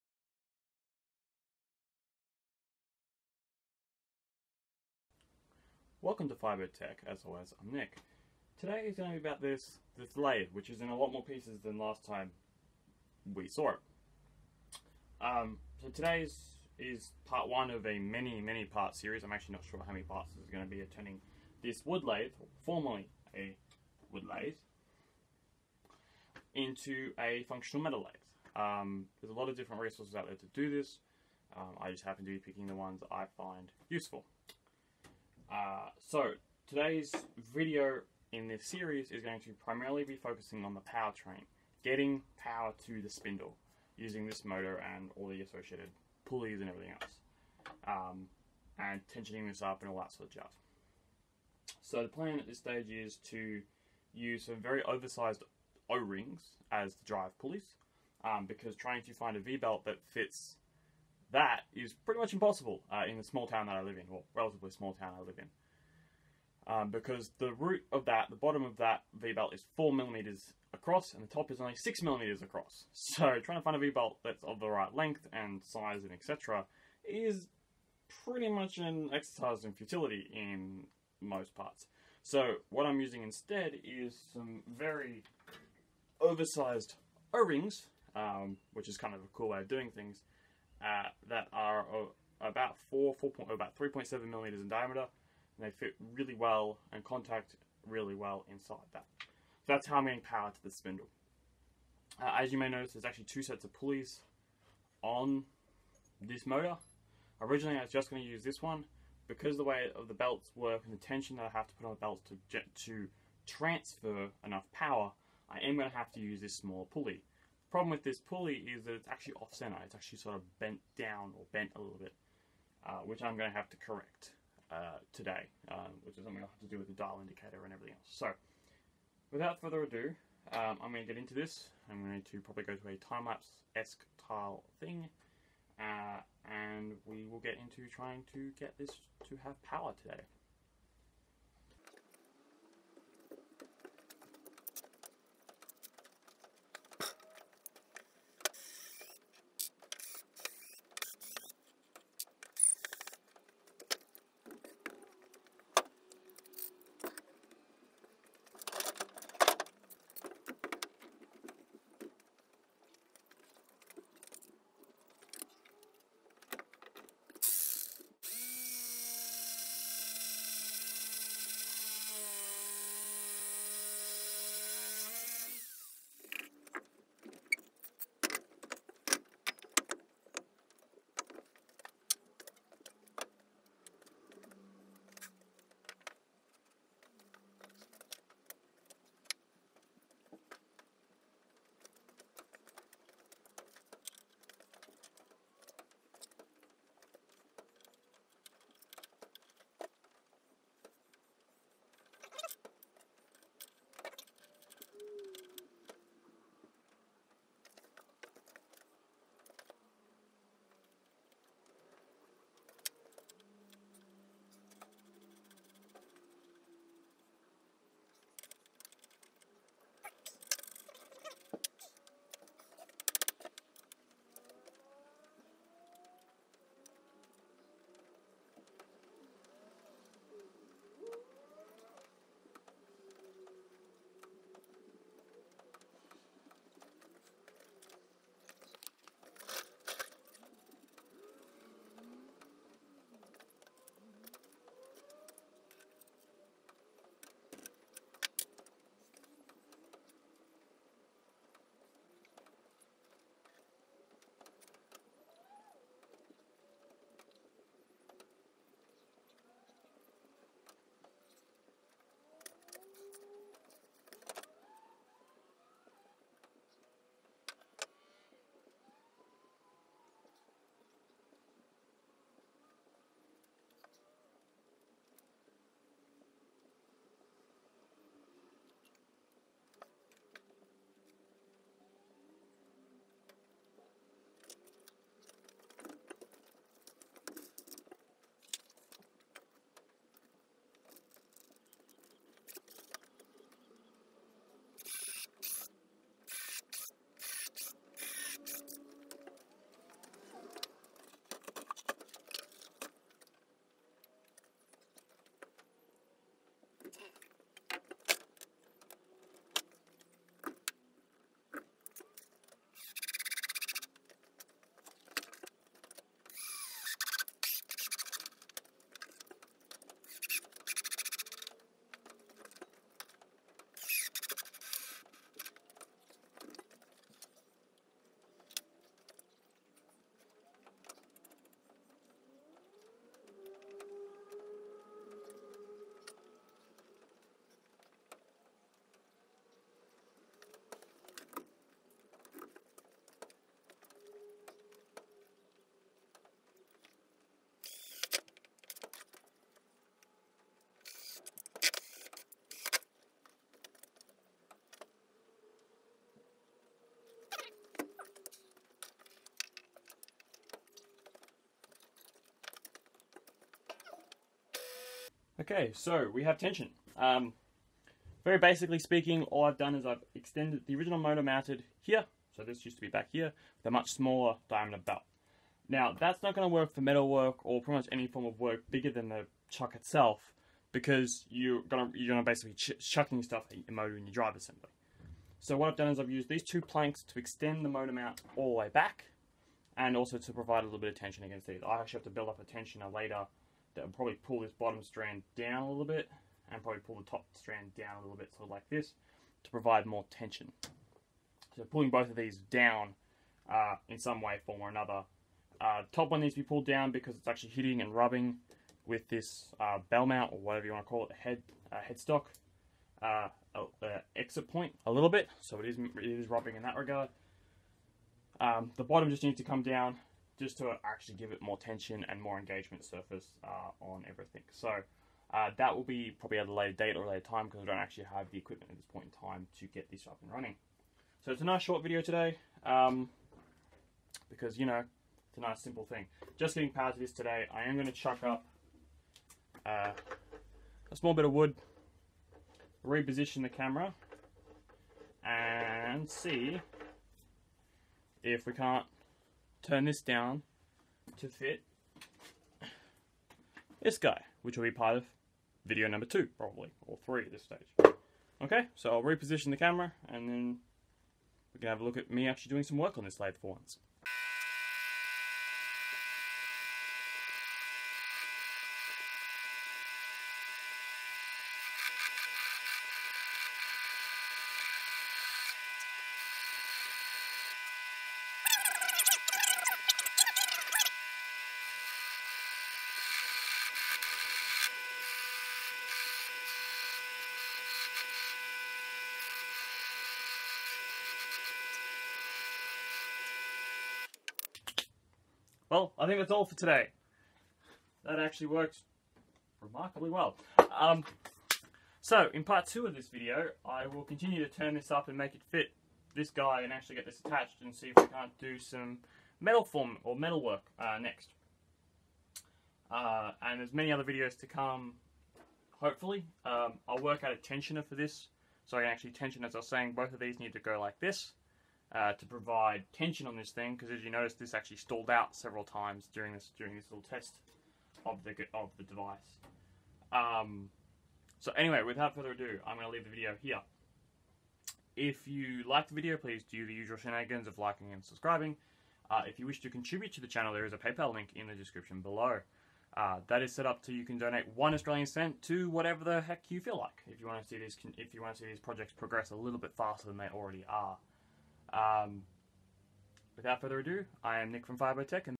wood lathe to metal lathe part 1
stage one of turning a wood lathe into a metal lathe which is me getting power to the spindle